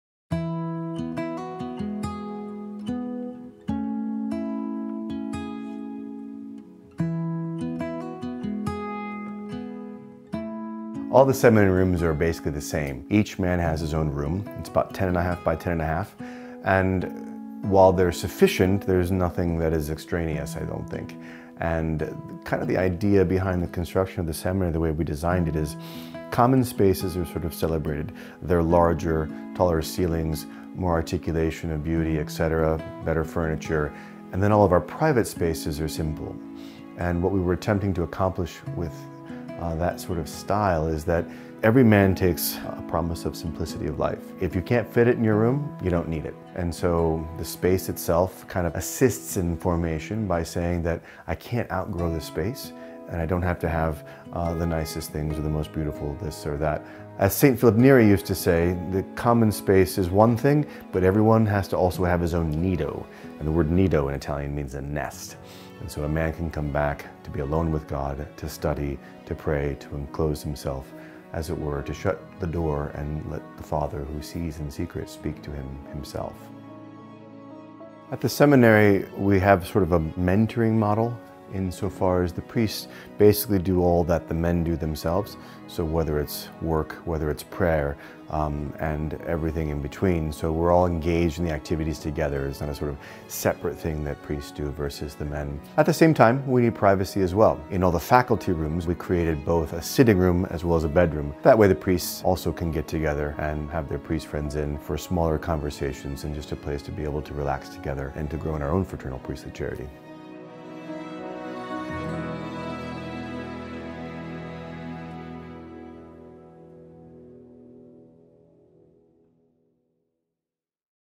In These Hallowed Halls - The Nido
The ideal seminary environment is designed to assist in the formation of the “whole man.” It should form the spirit and turn the soul toward God. As such, we searched for property that was quiet, remote, and removed from the noise of secular society—fertile ground for the seed of a priestly vocation to grow.

In April of 2020, we were able to start moving into the newly built seminary. We emptied four houses worth of belongings and put them in their now permanent locations. The Echo class was the first incoming class to be welcomed into the new seminary building, where they are for the first time living in community with the other three classes under one roof.

Here is a walk-through of the new seminary (video 4 of a 6 part series)